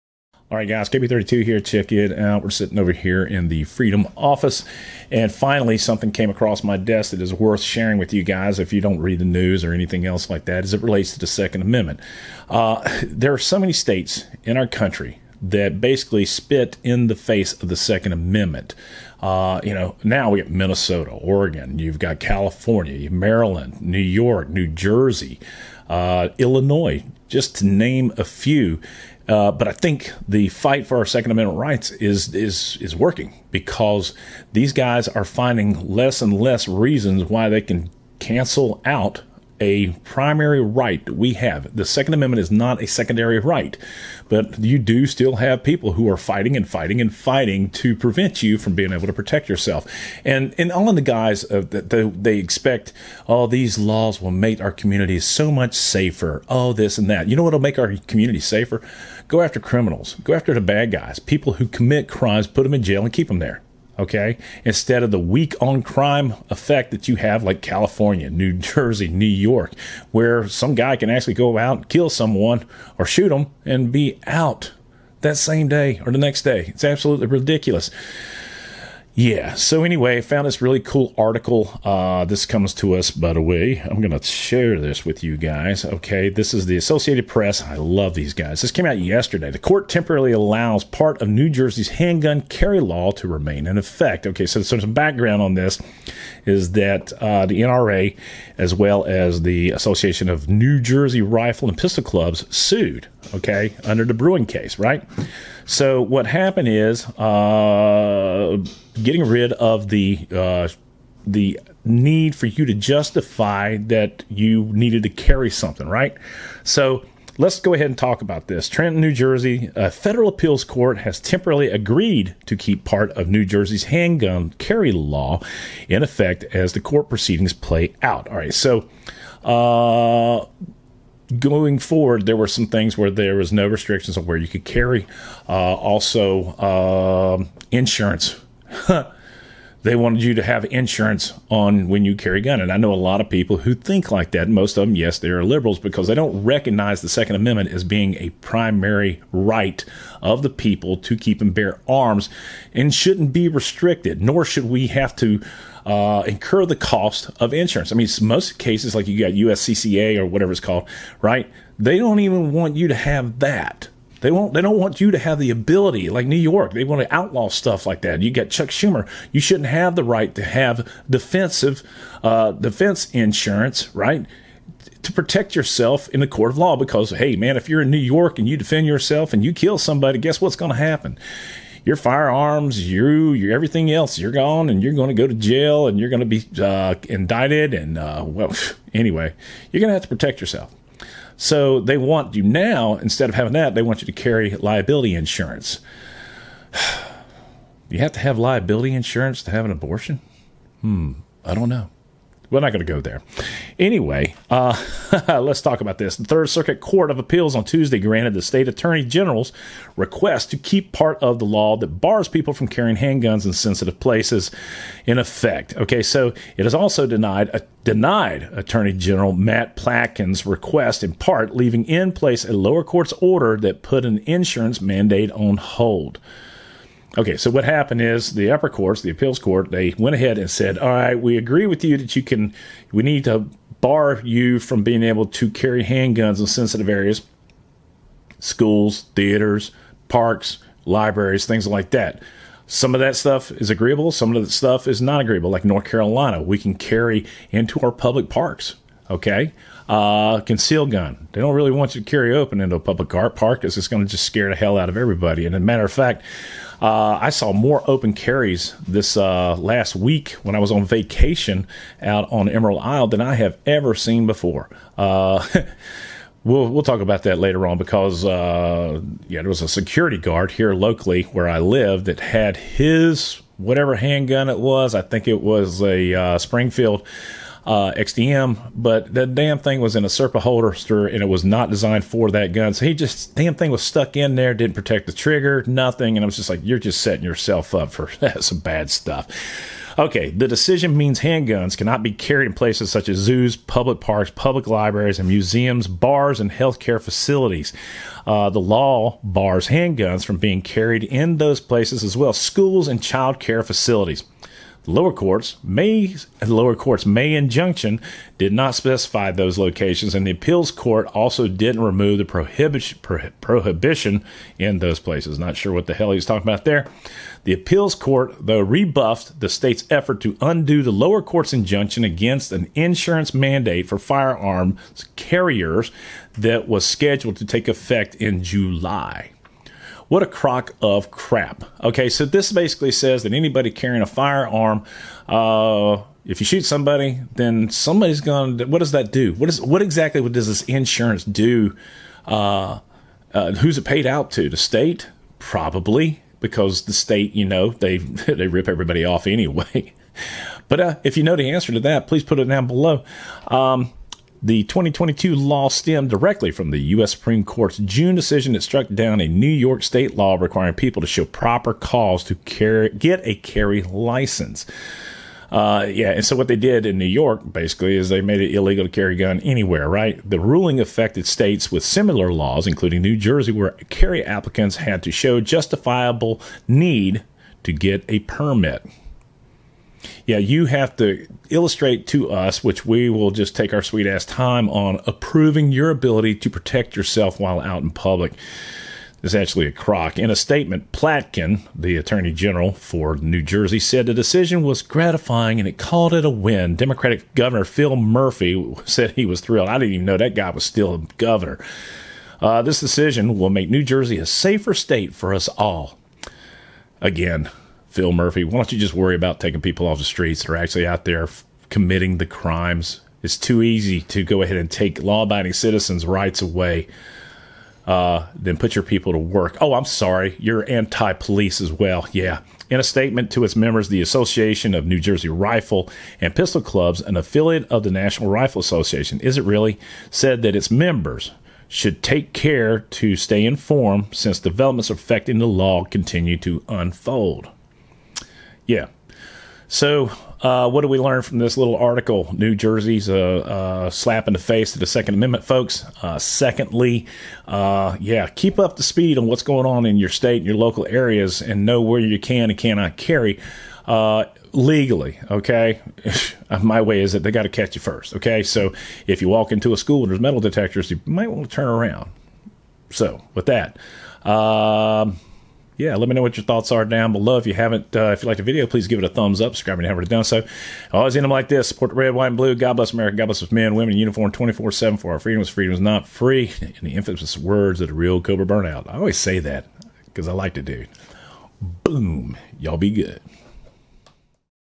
New Jersey's Insurance Mandate on Firearms Fails in Appeals Court
Sensitive places and Insurance Mandate partly fails, halting the Insurance Mandate.